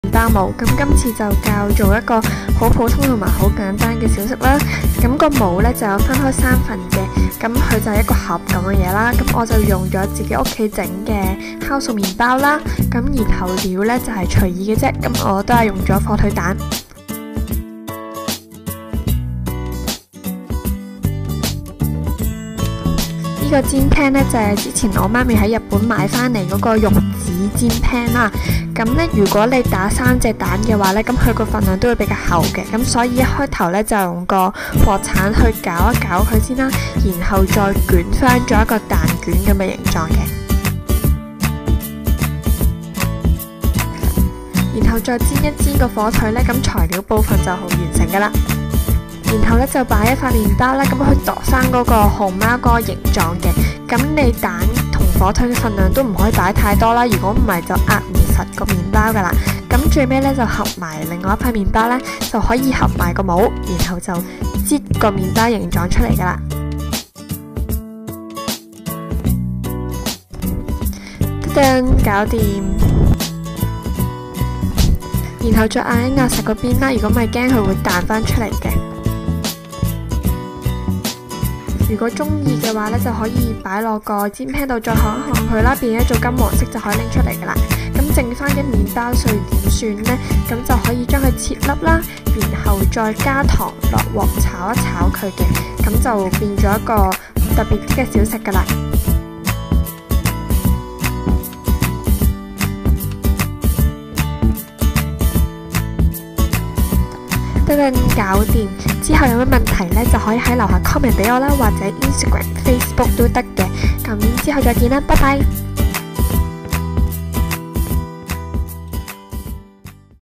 極速麵包小食 My Easy Breadsnacks Tutorial | Neat Freaks Diary
其實係替男朋友送早餐速遞，順便試用新買得麵包模